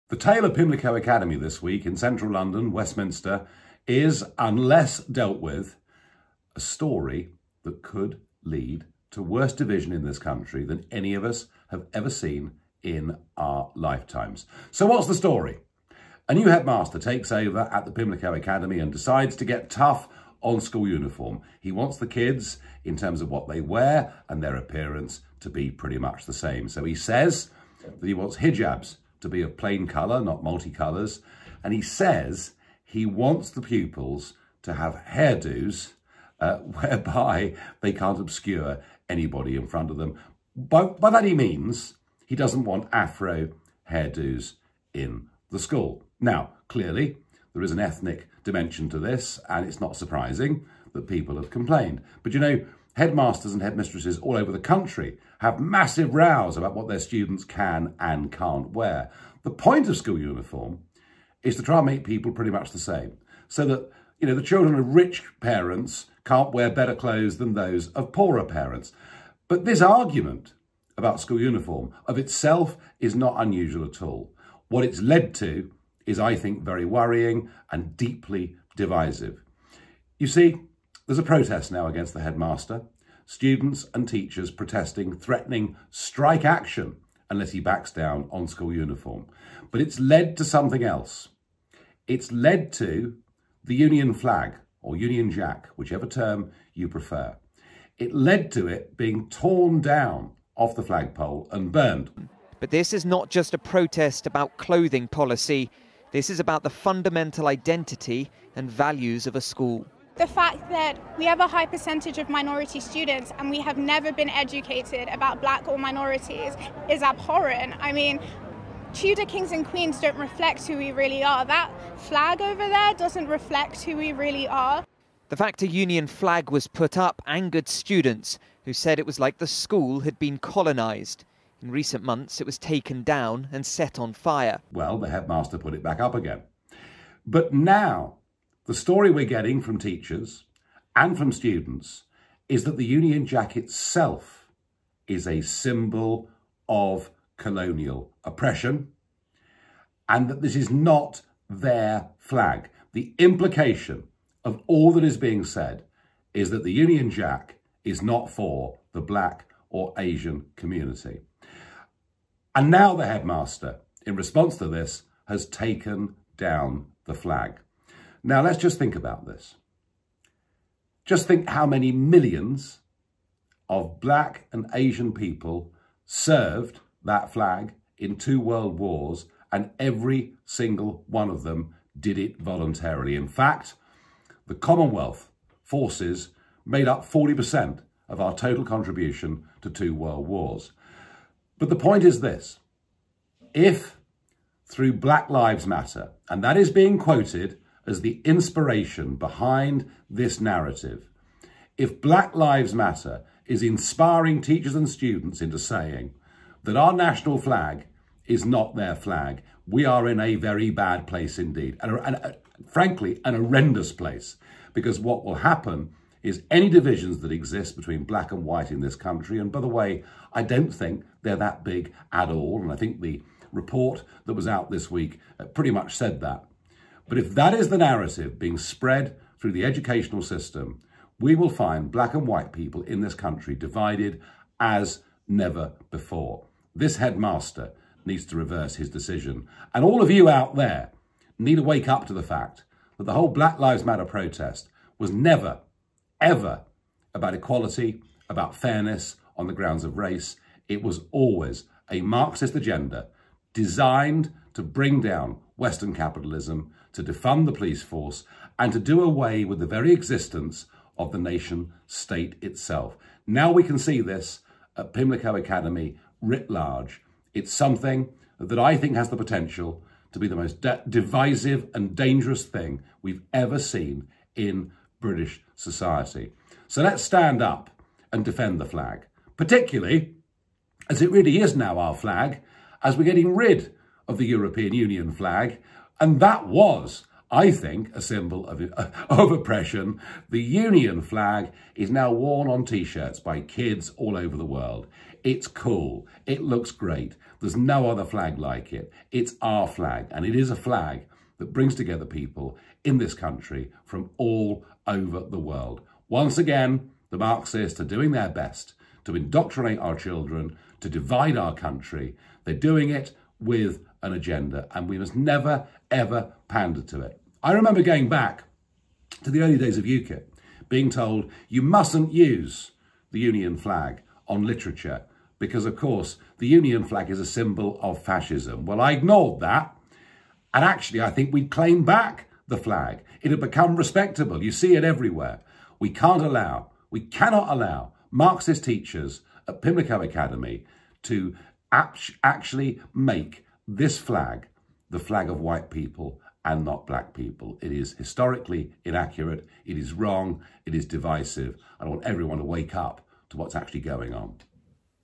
What is going on at Pimlico Academy?
This news story could be really bad news for division in this country.